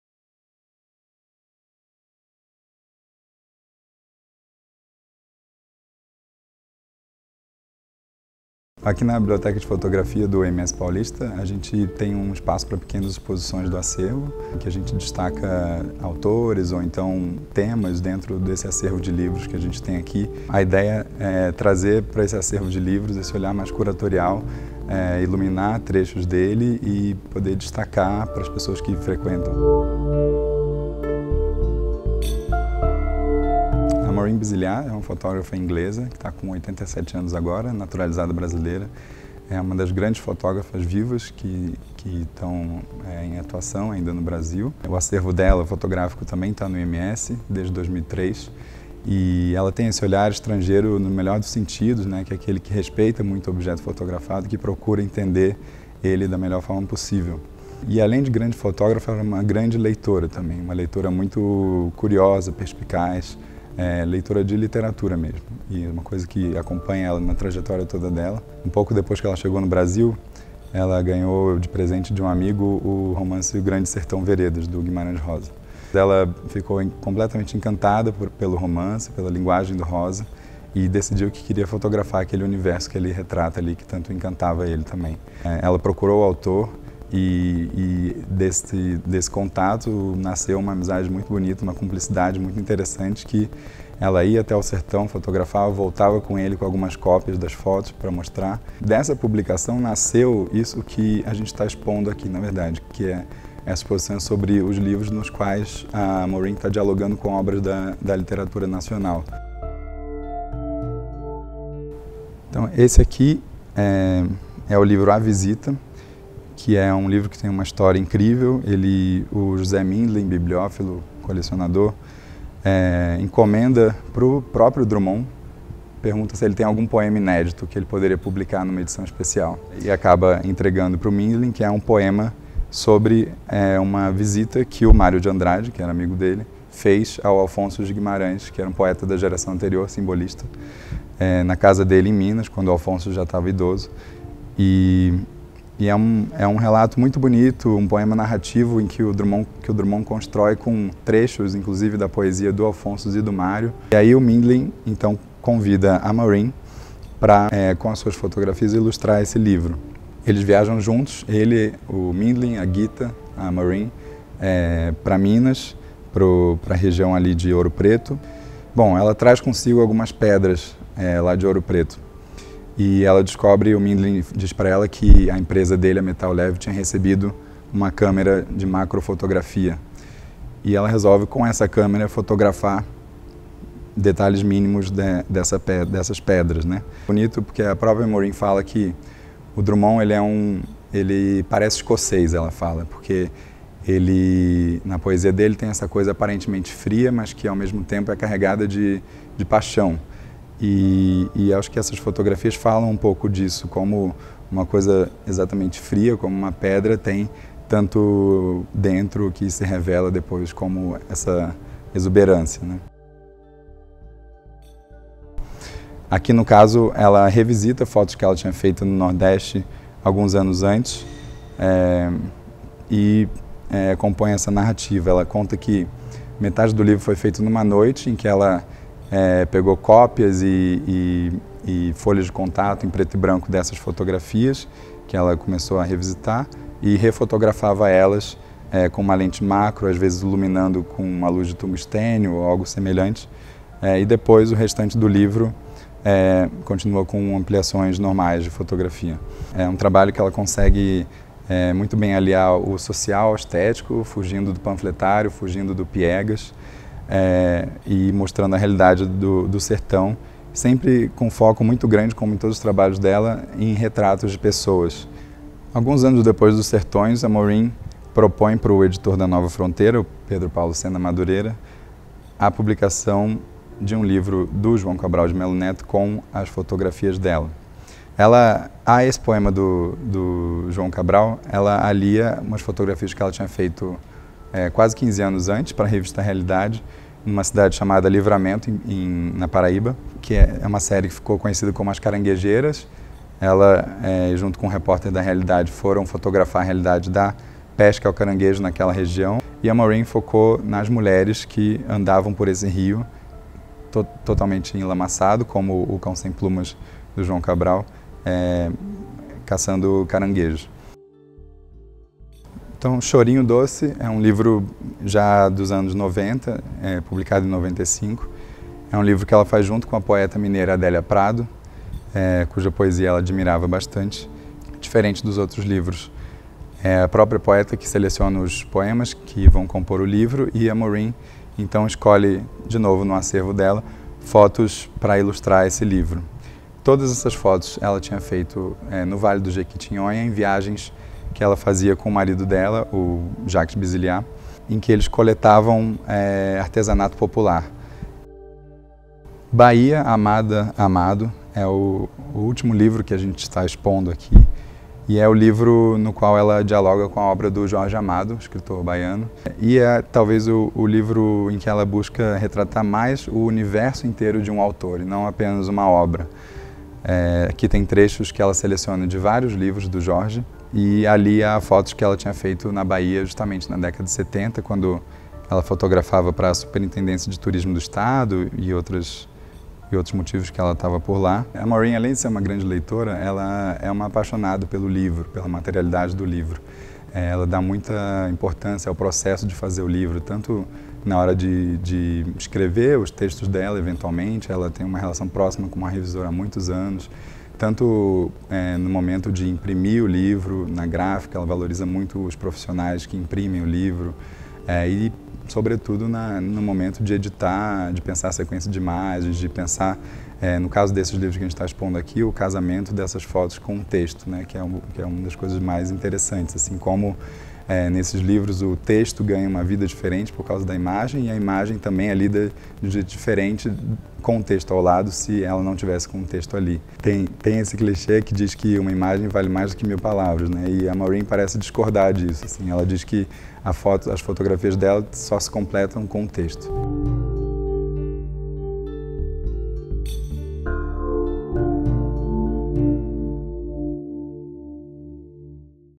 Visita à exposição "Fotografia e literatura nos livros de Maureen Bisilliat"
Miguel Del Castillo, curador da Biblioteca de Fotografia do IMS Paulista, faz uma visita guiada à exposição 'Fotografia e literatura nos livros de Maureen Bisilliat', uma celebração do casamento entre imagens da artista e textos de autores consagrados.

Poucos fotógrafos traduziram tão bem o Brasil em sua complexidade como Maureen Bisilliat. Esta exposição destaca seis livros em que suas imagens dialogam com a obra de grandes escritores da literatura nacional: João Guimarães Rosa, Carlos Drummond de Andrade, Euclides da Cunha, João Cabral de Melo Neto, Adélia Prado e Jorge Amado – autores que, como ela, oferecem visões incontornáveis da alma brasileira. Leitora perspicaz e curiosa, Bisilliat parece discordar do clichê de que uma imagem vale mais do que mil palavras: “Minhas fotografias”, costuma dizer, “só ficam completas com o texto”.

Vídeo de Maria Clara Villas.